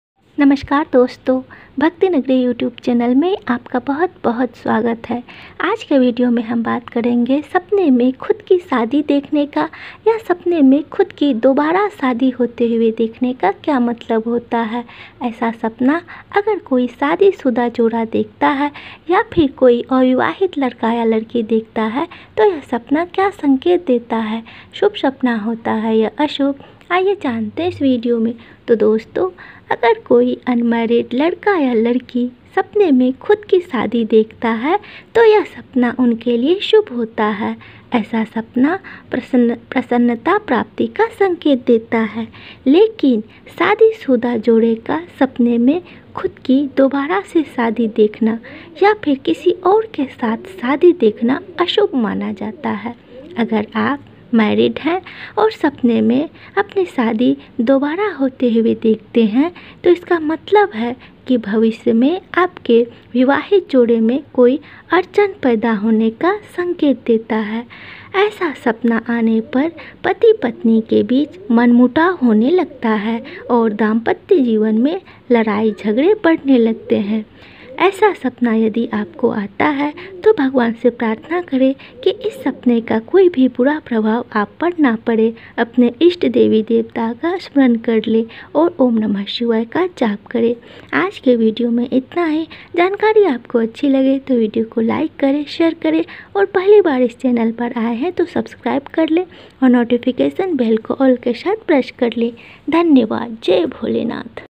सपने में खुद की शादी होते देखना l Sapne me khud ki shadi dekhna.
सपने में खुद की शादी होते देखने का क्या मतलब होता है अगर कोई शादी शुदा लोग ऐसा सपना देखे या कोई अविवाहित ऐसा सपना देखे तो इसका क्या मतलब है l

Sapne me shadi dekhna
Sapne me khud ki shadi dekhna
sapne me khud ki shadi hote dekhna
Sapne me shadi
Sapne me
Bhakti nagri